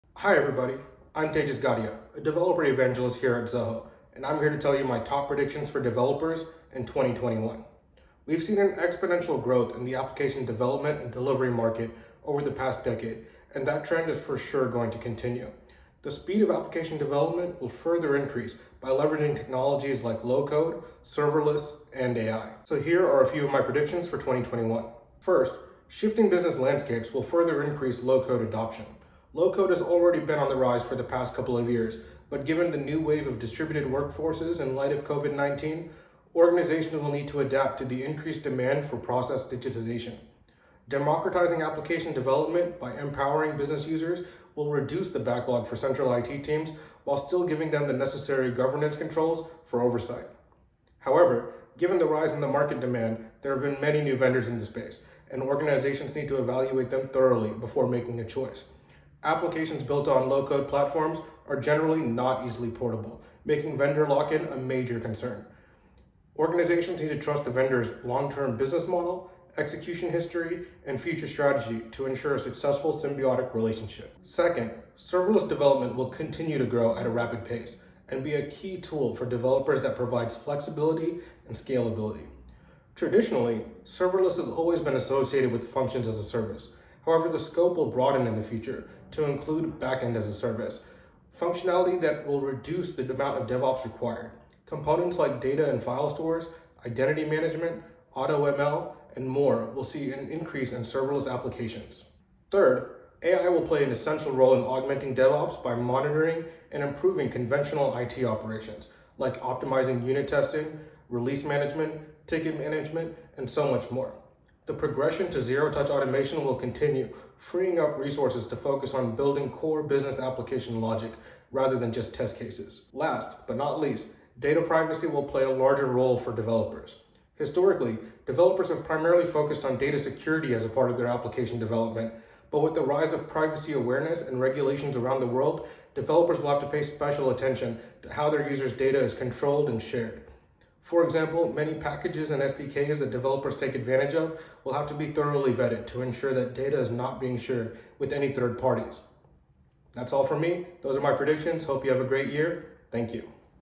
Top DevOps predictions for 2021
Our DevOps expert reveals their top predictions for developers in 2021. In the next year, low-code adoption will rapidly increase due to shifting business landscapes, serverless development will continue to grow, and AI will play a role in monitoring and improving IT operations.